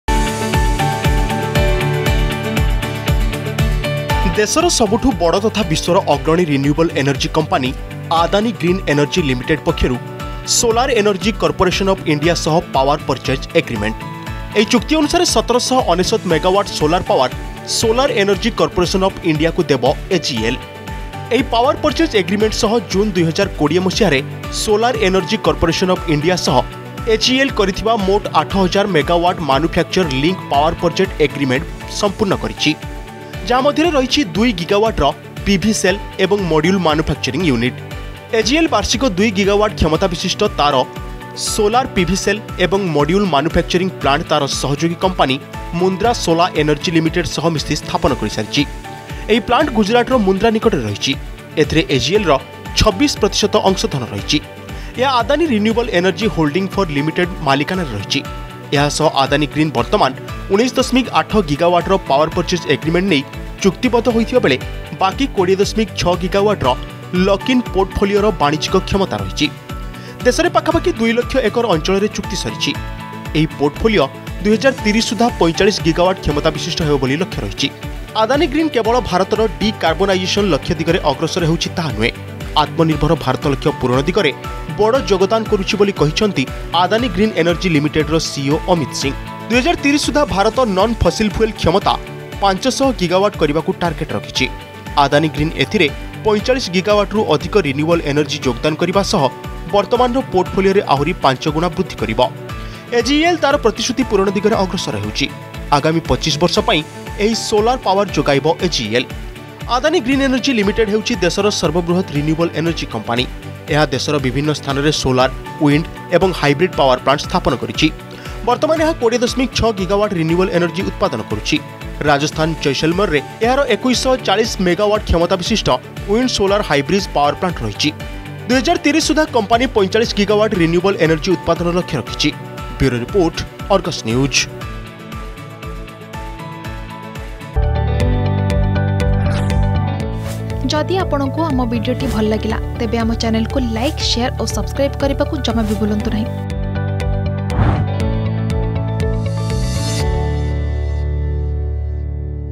ଆଦାନୀ ଗ୍ରୀନ ଏନର୍ଜି ଓ ସୋଲାର୍ ଏନର୍ଜି କର୍ପୋରେସନ୍ ଅଫ୍ ଇଣ୍ଡିଆ ସହ ଏଗ୍ରୀମେଣ୍ଟ || Adani Green Energy
ଆଦାନୀ ଗ୍ରୀନ ଏନର୍ଜି ଲିମିଟେଡ ପକ୍ଷରୁ ସୋଲାର୍ ଏନର୍ଜି କର୍ପୋରେସନ୍ ଅଫ୍ ଇଣ୍ଡିଆ ସହ ହୋଇଛି ପାଓ୍ବାର ପର୍ଚେଜ୍ ଏଗ୍ରୀମେଣ୍ଟ । 1799 ମେଗାଓ୍ବାଟ୍ ସୋଲାର ପାଓ୍ବାର, ସୋଲାର ଏନର୍ଜି କର୍ପୋରେସନ୍ ଅଫ୍ ଇଣ୍ଡିଆକୁ ଦେବ ଏଜିଇଏଲ୍ ।